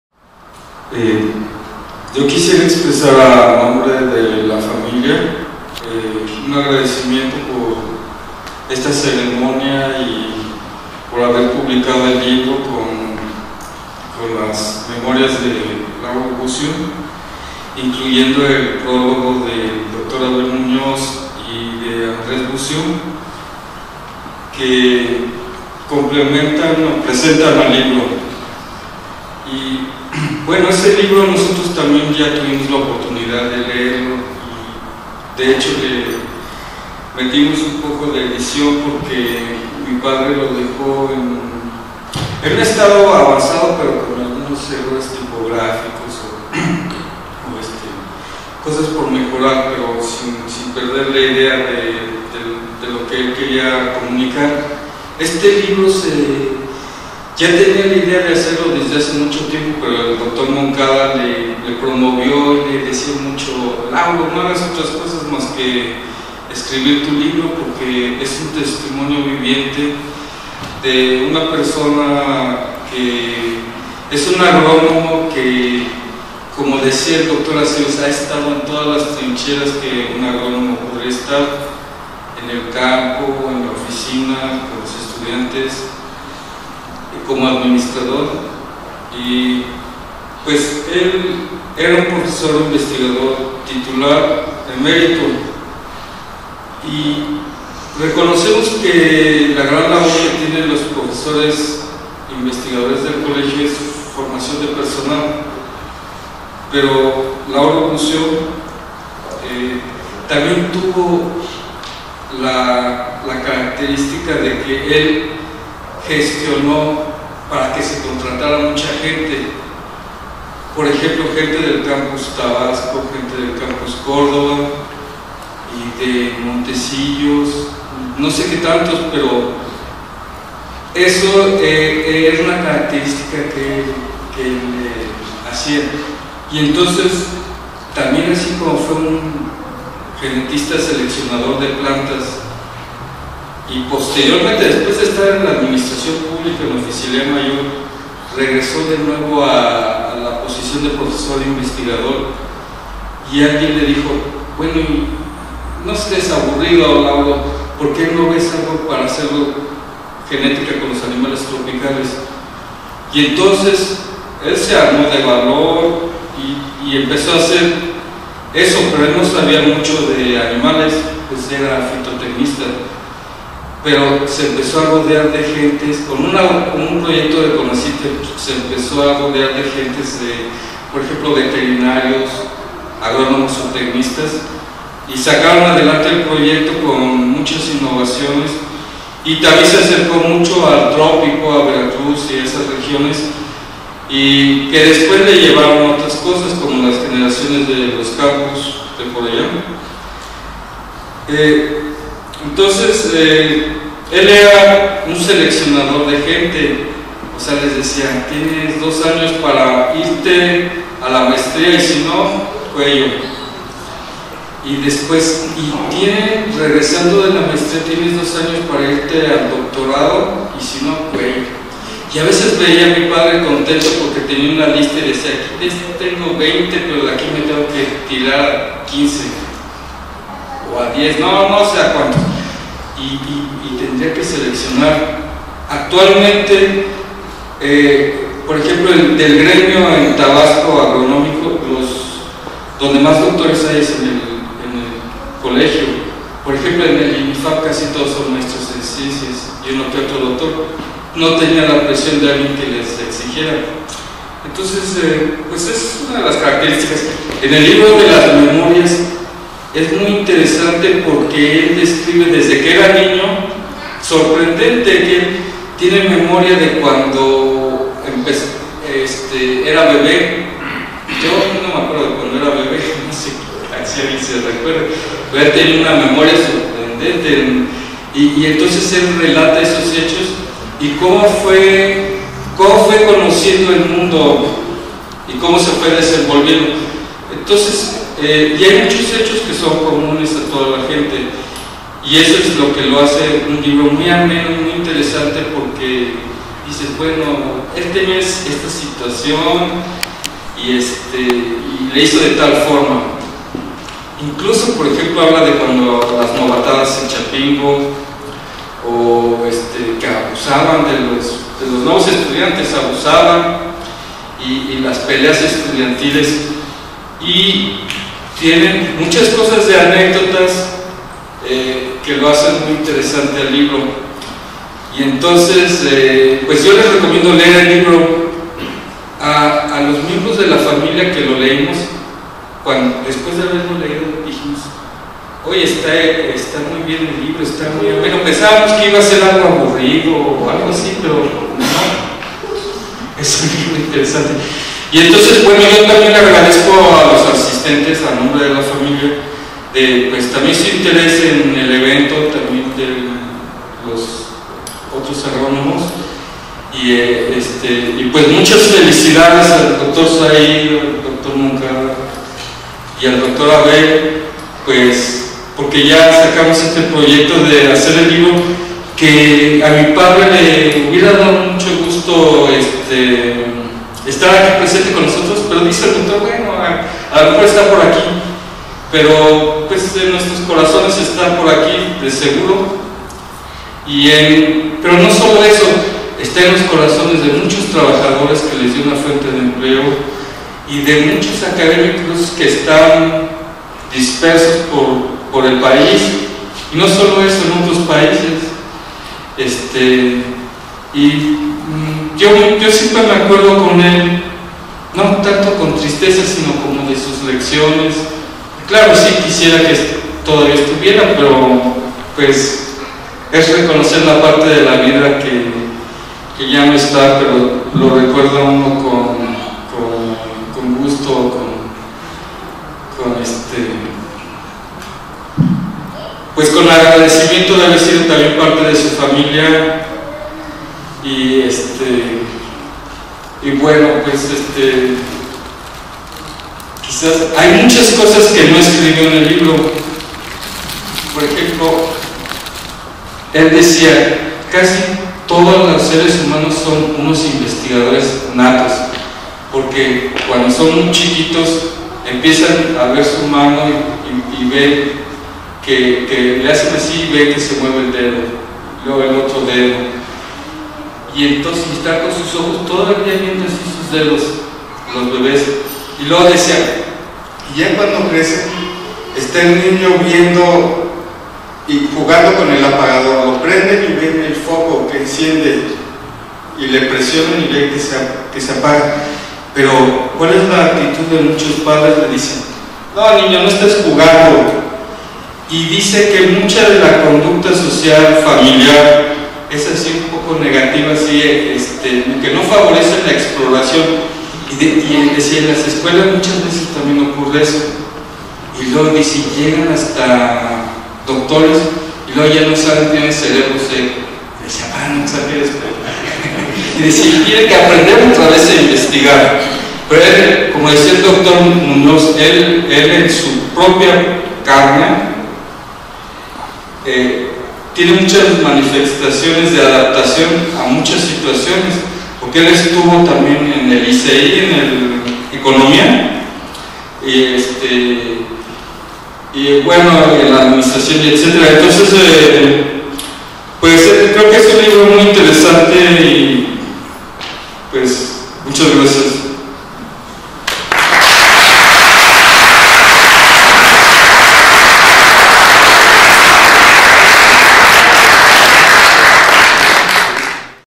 PALABRAS DE ADOLFO BUCIO: MEMORIAS DE LAURO BUCIO ALANÍS
Estas son las palabras del Dr. Adolfo Bucio Galindo en la presentación de las memorias del Dr. Lauro Bucio Alanís, durante la Feria del Libro de Guadalajara. 25 de noviembre de 2017.